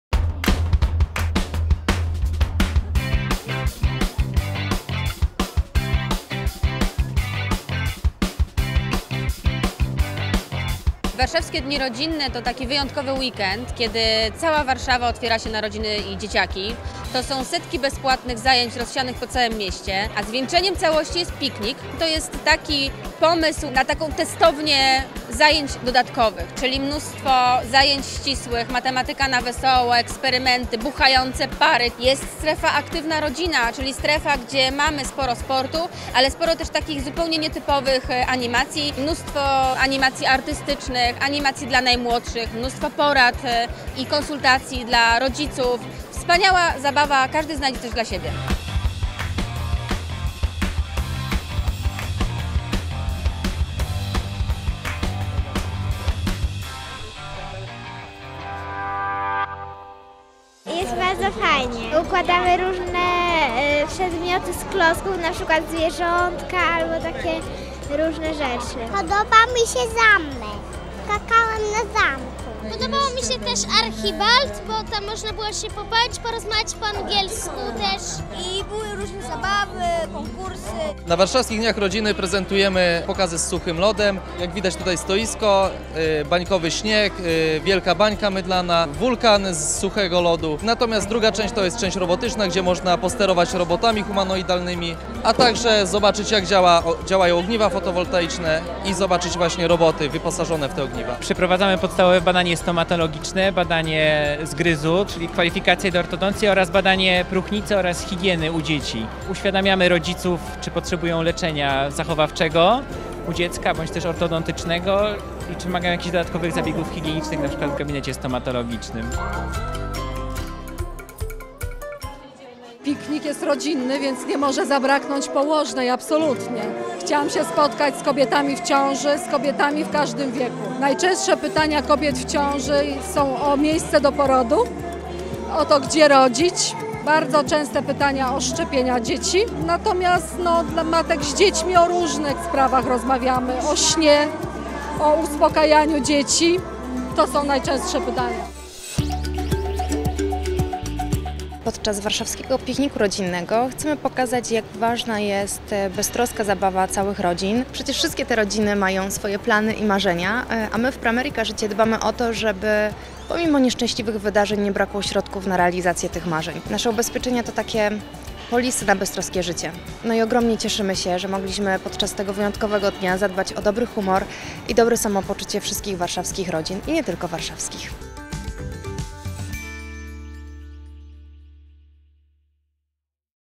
Moc atrakcji podczas Warszawskiego Pikniku Rodzinnego. X-NEWS.PL
Sztuczki magiczne, klown, eksperymenty naukowe, gokarty, czy wyścigi robotów – to tylko niektóre z atrakcji, z których można było skorzystać podczas Warszawskiego Pikniku Rodzinnego. Dni podczas których miasta zmienią się w place zabaw odbywać będą się do września w wybranych miejscach w Polsce. Wszystko po to, żeby pokazać rodzicom przyjazne dla nich i dzieci miejsca w ich miejscach zamieszkania.
W Warszawie można było zobaczyć m.in. wyjątkową strefę dla dzieci CHRONIĘ, BO KOCHAM. – To tutaj przyszli rodzice mogli porozmawiać o wsparciu fizycznym i psychicznym w okresie ciąży i połogu, a małe dzieci skorzystać z bezstresowej konsultacji stomatologicznej – mówi Małgorzata Morańska, rzecznik prasowy Pramerica Życie.